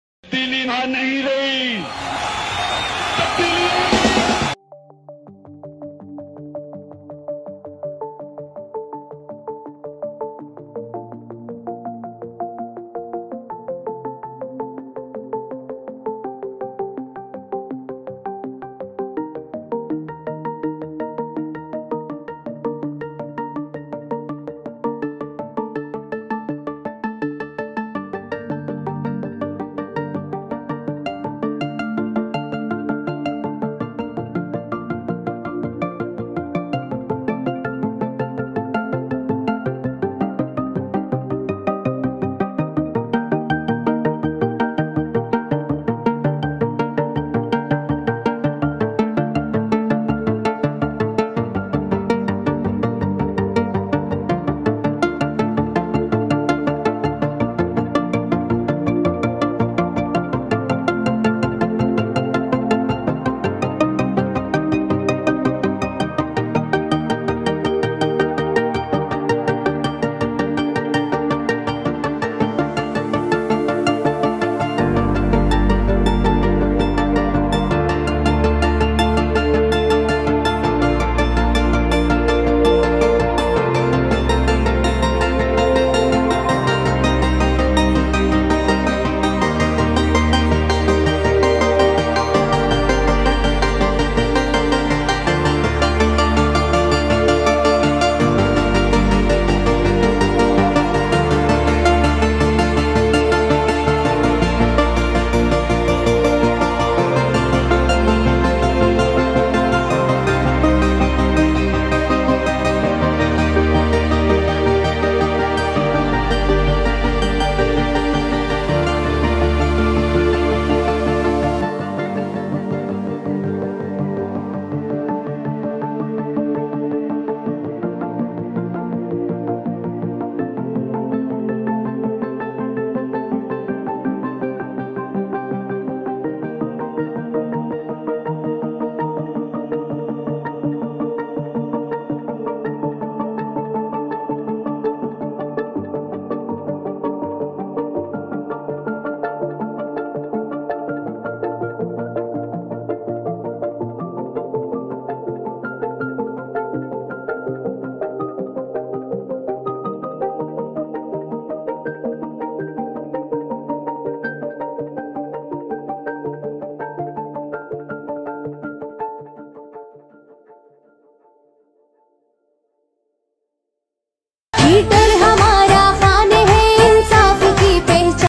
Sarai Saleha Degree College Haripur PTI Haripur Projects
One of the top Building in all over kpk constucted by PTI Govt. Govt Degree College Sarai Saleha Haripur
Project Amount is Rs: 3.3 Billion
Crddit goes to Khan Brothers